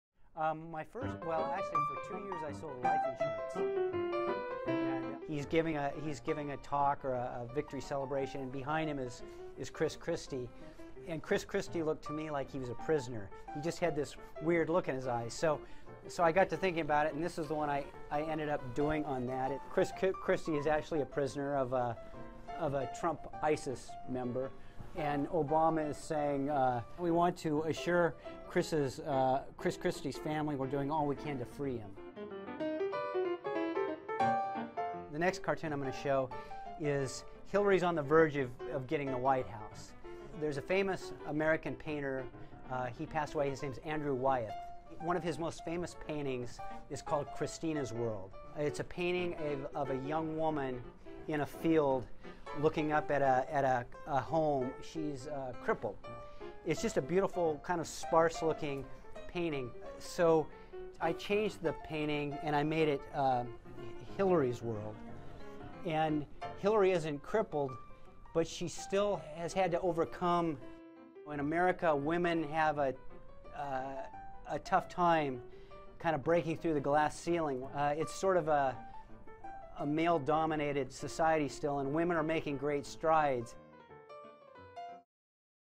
Mike Luckovich shows how to make a great political cartoon PT 3
There's a tradition in the United States called political satire. That's making light of what can sometimes be the rough and tumble world of politics. One of the most popular forms-- dating back hundreds of years-- is political cartooning. Mike Luckovich is one of the best in the U.S., having won the Pulitzer Prize. He showed CCTV America what it takes to make a great political cartoon.